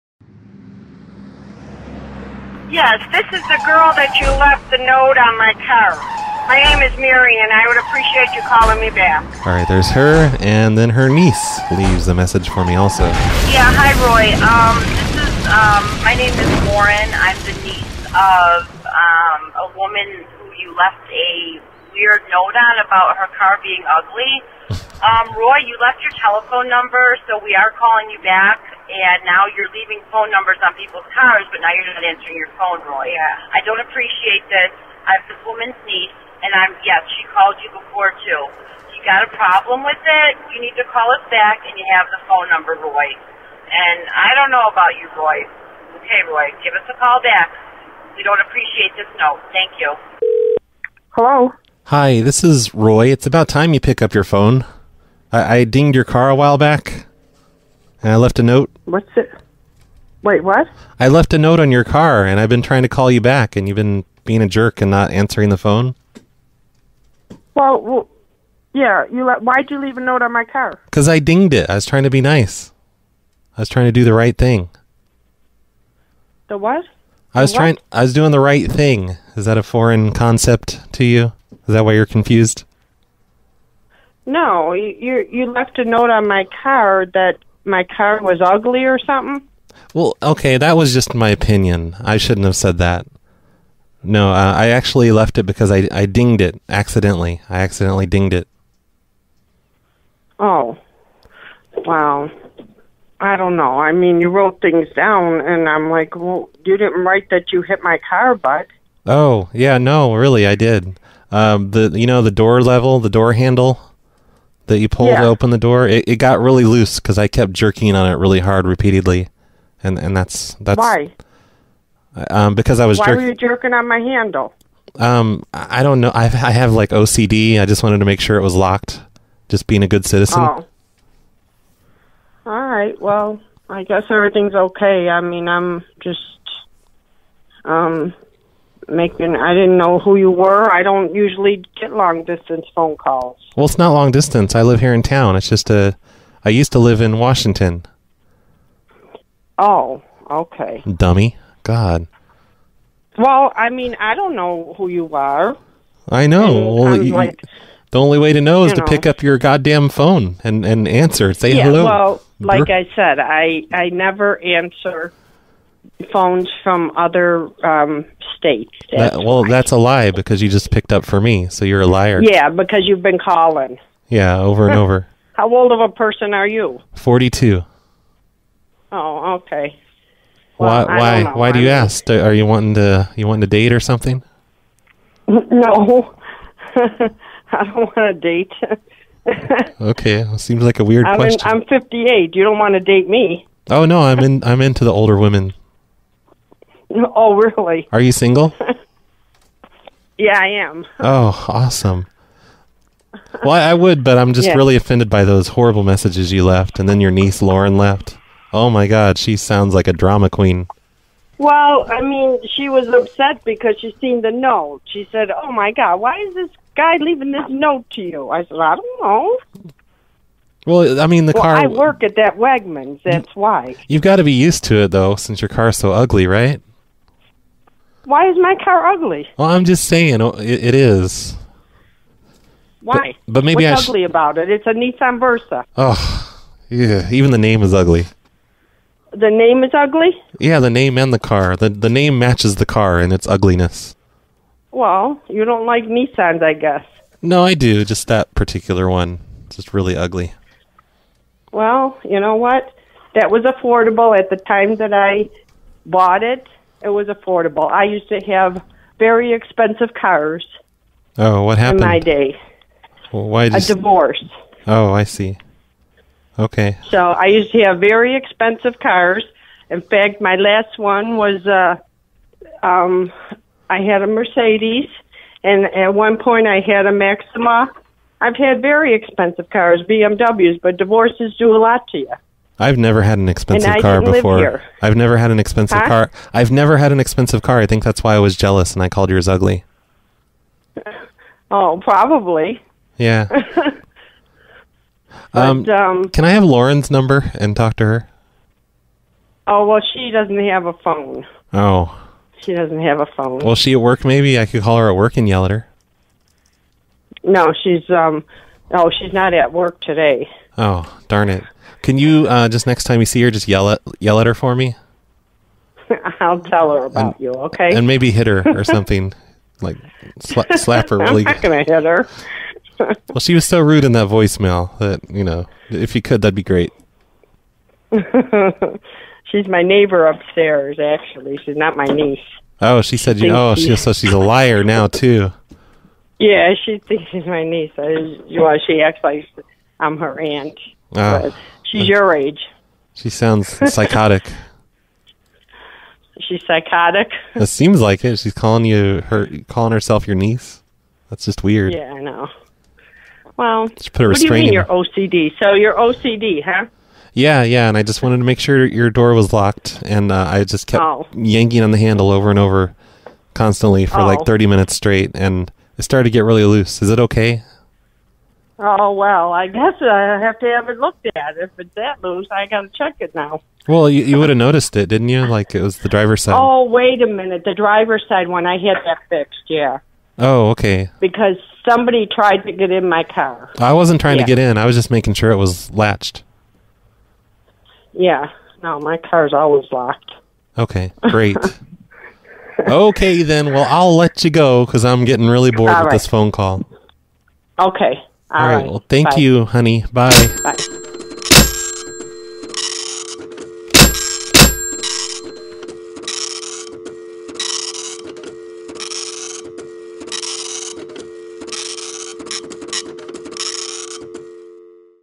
Sorry I Dinged Your Car - Why Were You Jerking On My Handle?
Listeners all over North America leave notes on car windows for me that read "Sorry I dinged your car. Please call me back at 401. -Roy." This causes people to call me and I get to deal with whatever problems the note causes. This is something we only do in September of each year, and the number changes every year, so don't leave notes on cars with the phone number in this video because it won't work after September 2016.

You'd expect this lady to yell at me a bunch once you hear the voicemail she left for me, but instead she mostly just makes me feel bad for bothering her.

I completely forgot about making fun of her specific model of car several months later as I was buying the exact same one.

That really neat animated car intro was done by Carmen.